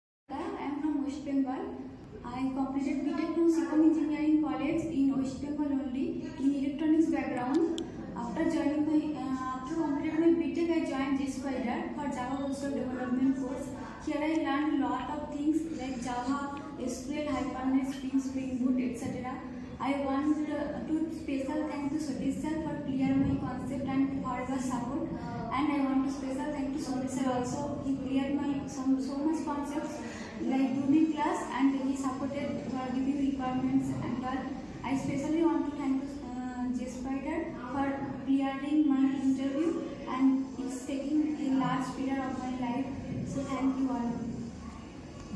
Rajeshwari Dutta's Path to Software Engineering Excellence.
escription: Explore Rajeshwari Dutta's incredible journey from the circuit branch to becoming a skilled Software Engineer with JSpiders! Despite starting with no knowledge of Java, Rajeshwari now commands strong expertise in Java, SQL, J2EE, Frameworks, and Web Technologies. Special gratitude to Sathish Sir for his exceptional support and expertise in Hibernate and Spring, and to Sameer Sir. Rajeshwari sincerely appreciates JSpiders for providing excellent training that has made a significant impact on her life.